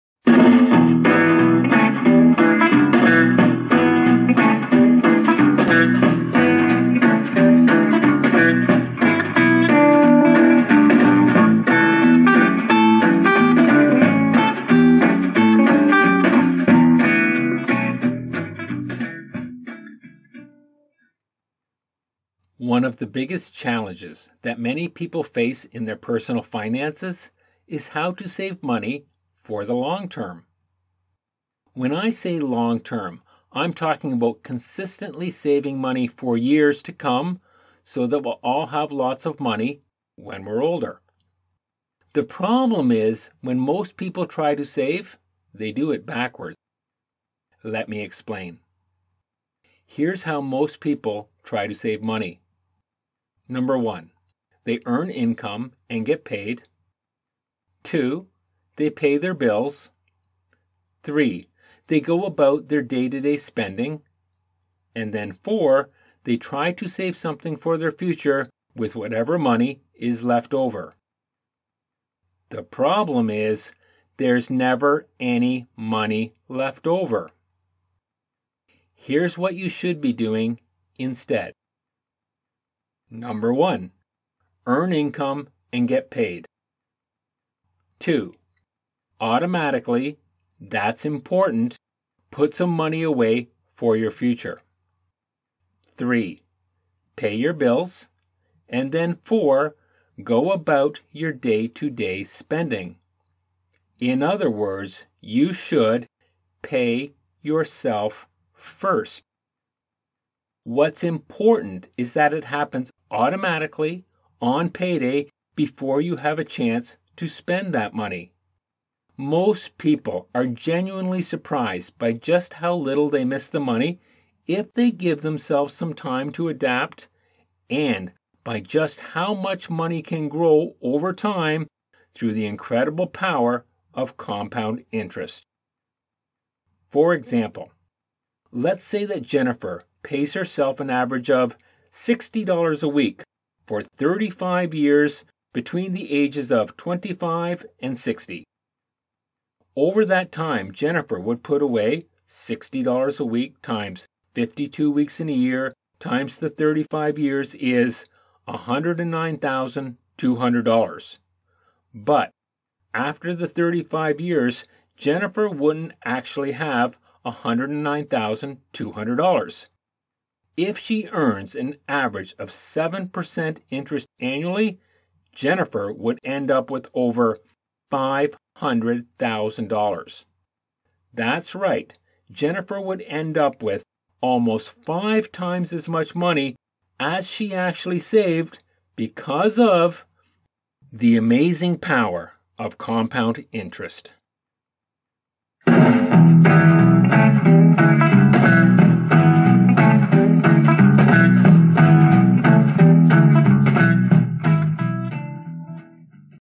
Money Mini #18 Pay yourself first
From the Author of Wealthing Like Rabbits: An Original Introduction to Personal Finance Author Robert R. Brown has now created a mini video series aimed at helping students manage their money better.

We here at AUSU have partnered with Robert Brown to bring you these fun videos with tips and tricks to help you better understand and manage your finances.